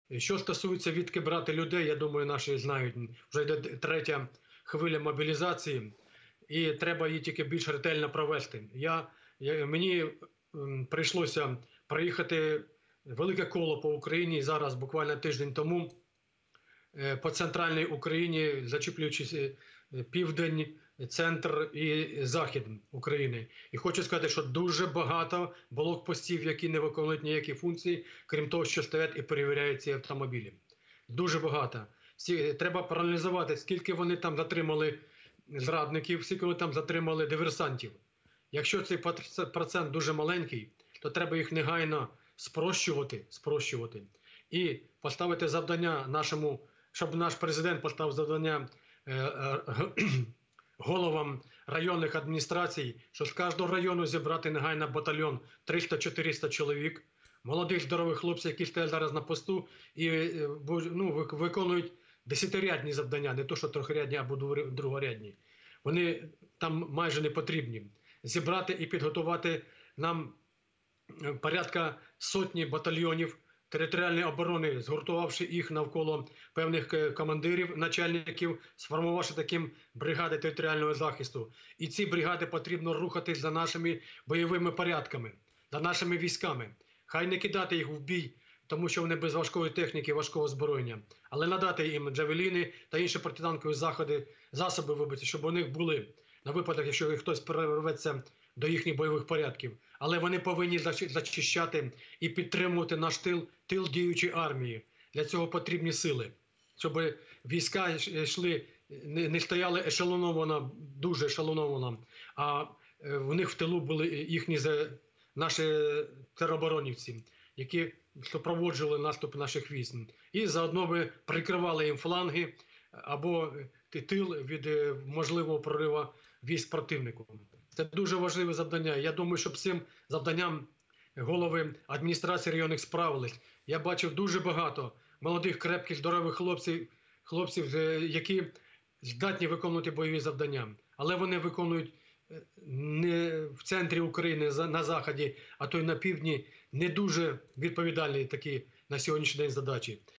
ТЕРОБОРОНА ПОВИННА ЙТИ НА ФРОНТ ❗️ ЧИ Є ЛЮДИ ДЛЯ КОНТРНАСТУПУ ЗСУ
В ефірі: Іван Якубець, військовий експерт